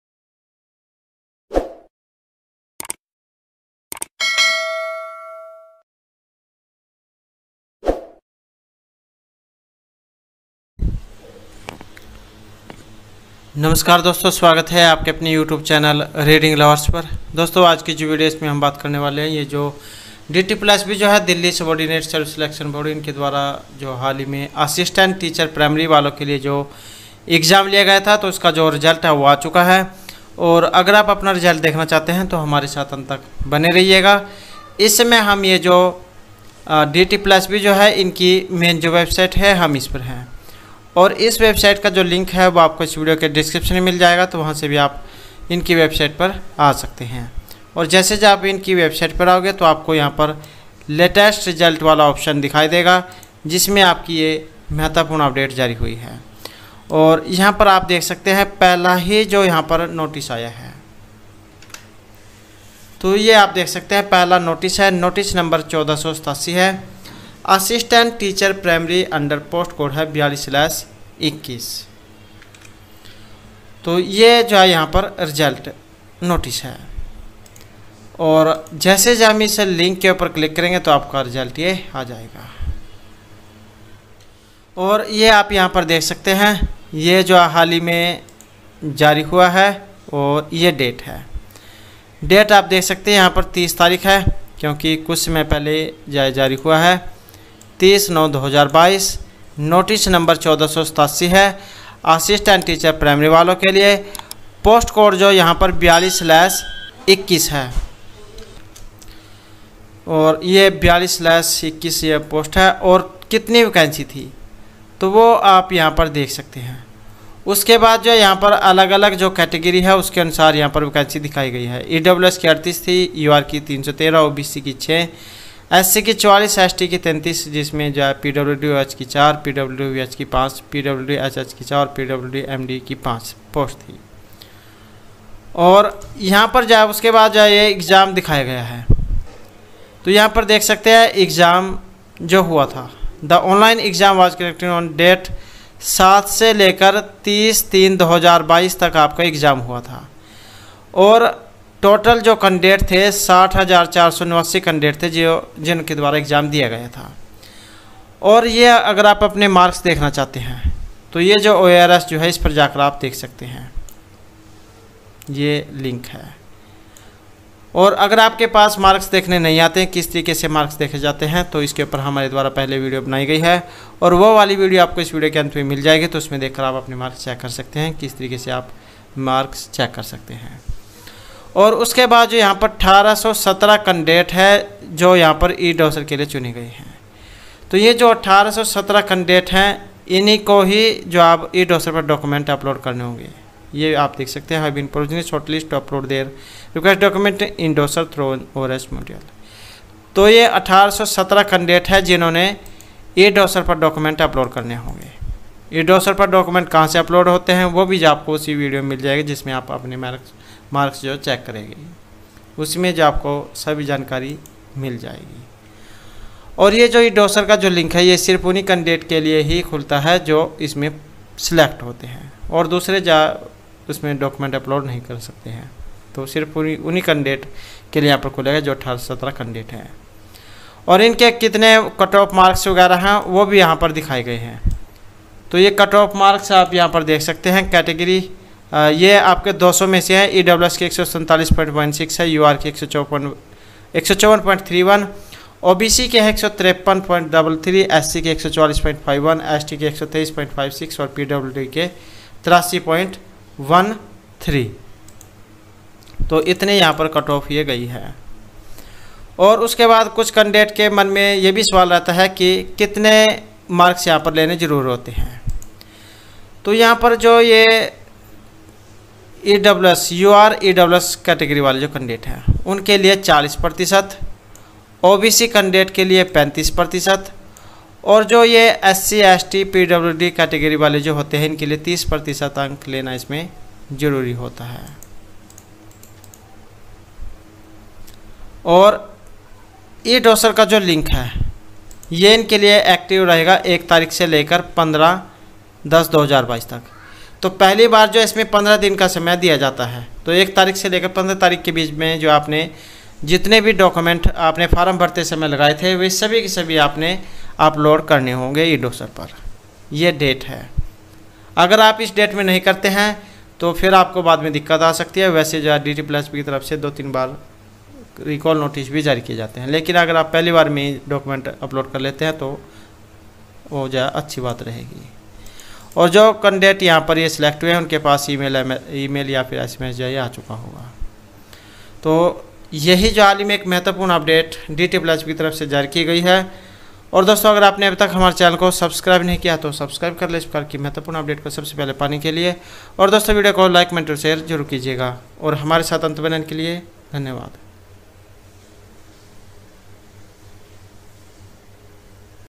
Dsssb Final Result & Cutoff Marks ASSISTANT TEACHER PRIMARY. DSSSB latest news today. DSSSB Result
In this video, we'll discuss the HOW TO DOWNLOAD DSSSB ASSISTANT TEACHER PRIMARY UNDER POST CODE 42/21 IN DOE. It's an amazing opportunity for all Indian candidates to get a government job in Delhi.

TAGS.